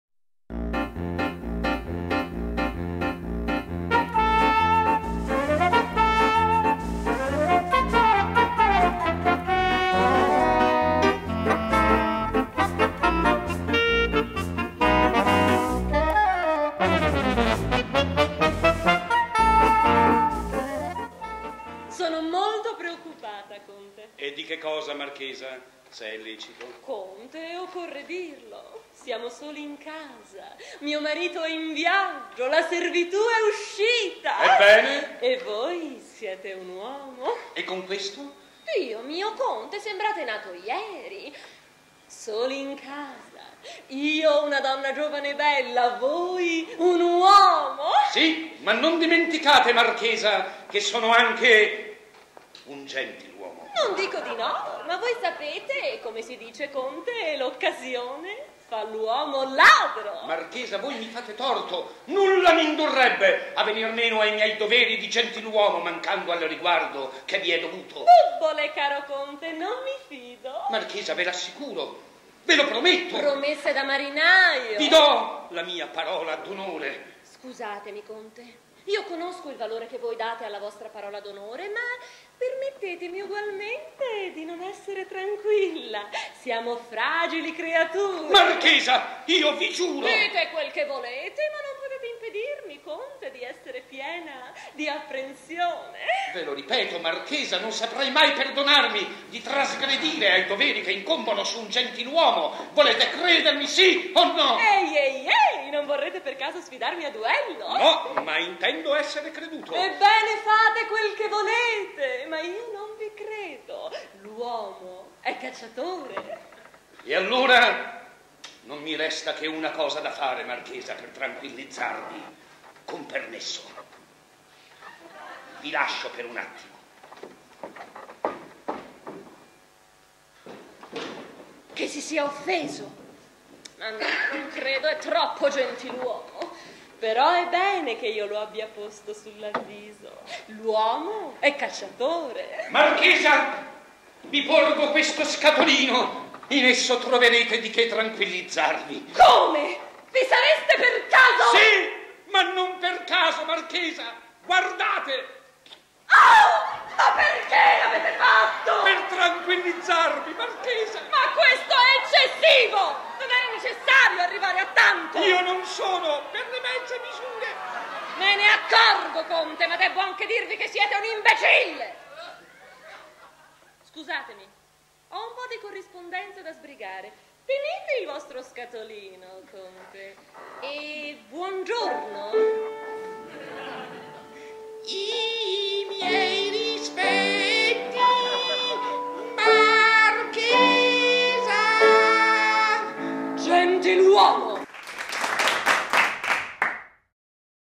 Achille Campanile: "Un gentiluomo"
Teatro La Contrada di Trieste, 1995: "Centocinquanta la gallina canta", testi di Achille Campanile, regia di Antonio Calenda, musiche di Germano Mazzocchetti. In questa scena un signore (Orazio Bobbio) utilizza una soluzione radicale per convincere una signora (Rita Charbonnier) di essere un vero gentiluomo.